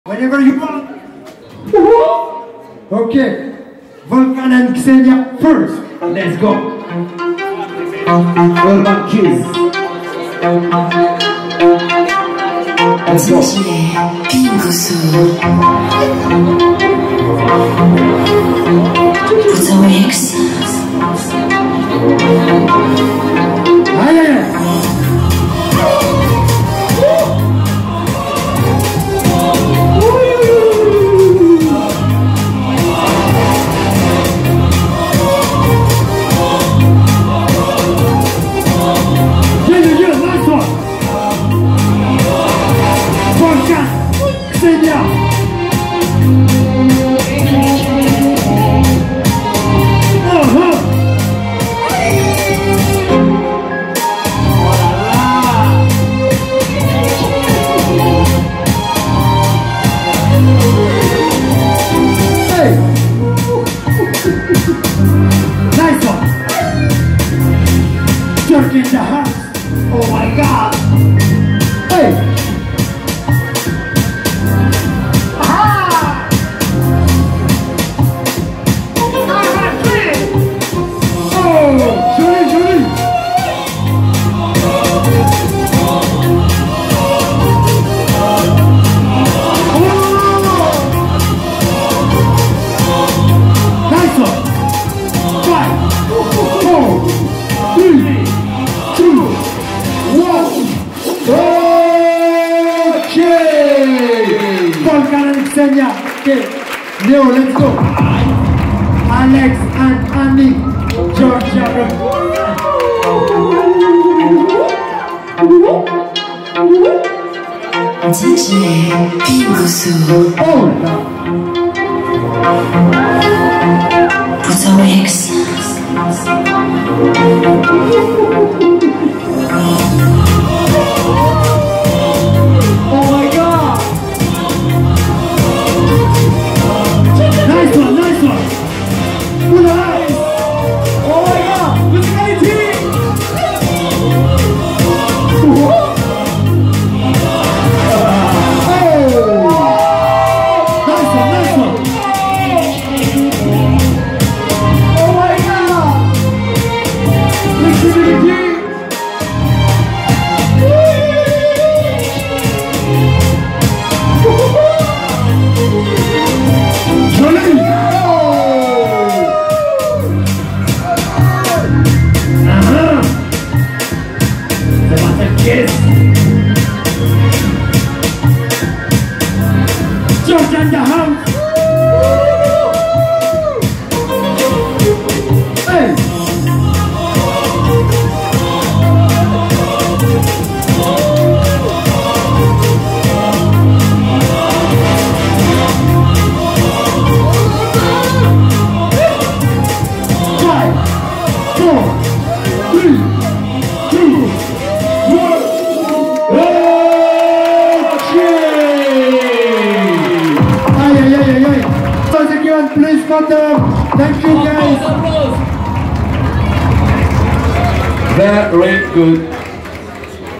Volkan & Xenia VS alexkaro & Annie @The Battle Kizz Congress World Final 2024 🙏@SaidDStreet🎵 Dj Neo
Volkan & Xenia VS @The Battle Kizz Congress ⚜️World Final⚜️ 2024
🎵 Live Songs by Dj Neo

🙏 Said DStreet ( The Battle Kizz )
🙏 Mou Oulad